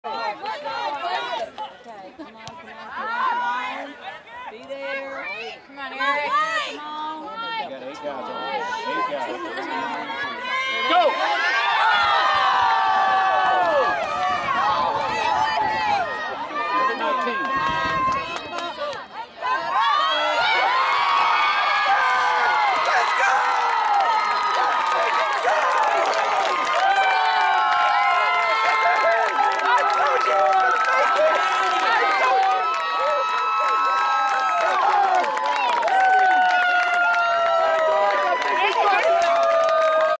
Kevin #12 redirect by Danny #3 to win in OT vs LeesMac 3 2 wow
Good teamwork in OT, with Kevin #12 shot redirected by #3 Danny for Win goal to beat a very good team in OT , and secure post-season play for Barton soccer